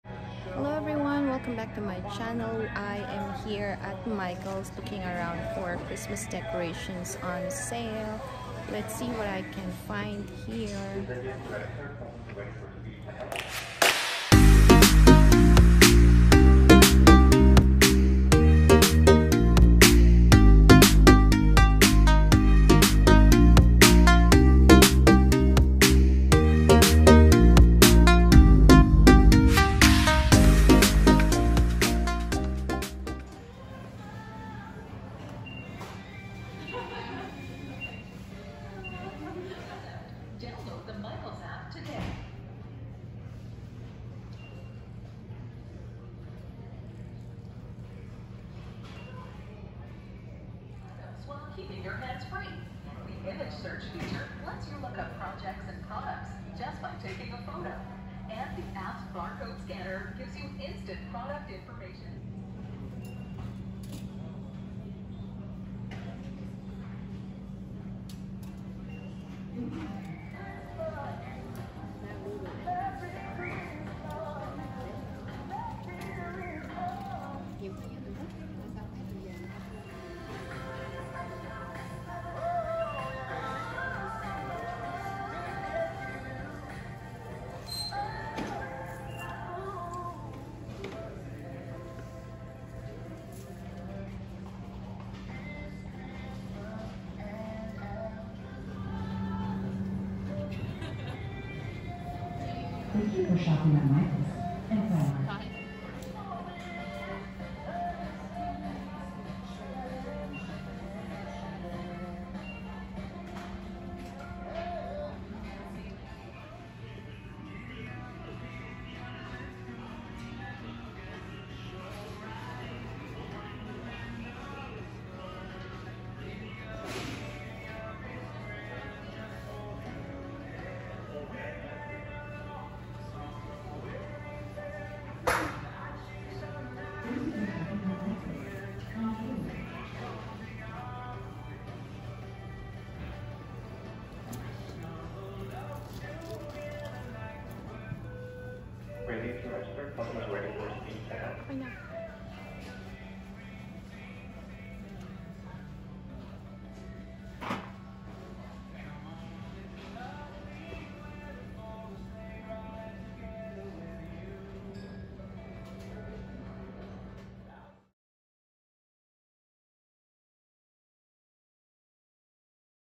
MICHAEL’S CHRISTMAS DECOR SALE | HAULS
This is the best time to buy Christmas decorations after the holidays for bargains. Michael’s has a huge store wide sale.